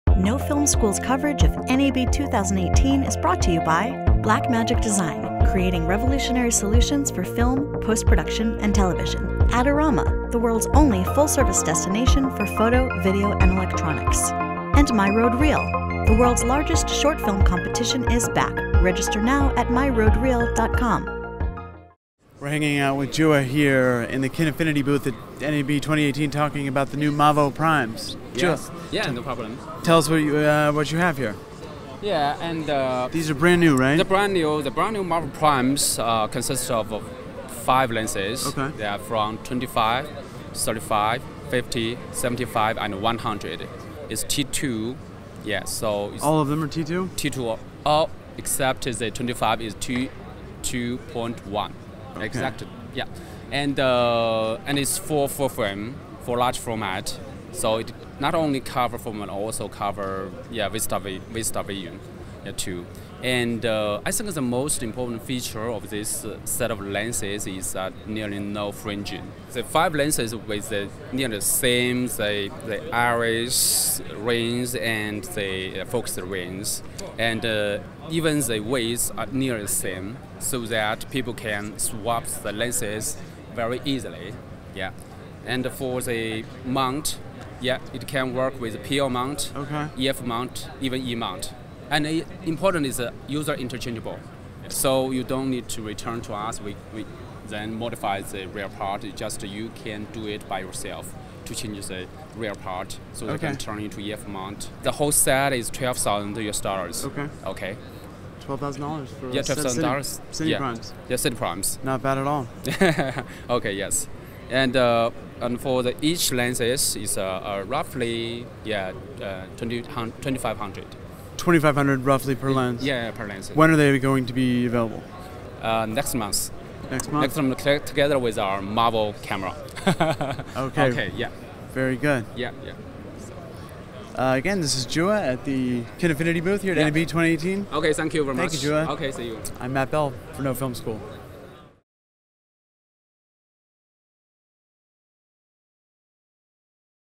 Looking for Affordable 6K Full-Frame Primes? Check Out Kinefinity
Kinefinity has a new option with their Mavo primes which offer full-frame sensor coverage, a T2 aperture, and an amazing price point of only $2500 a lens.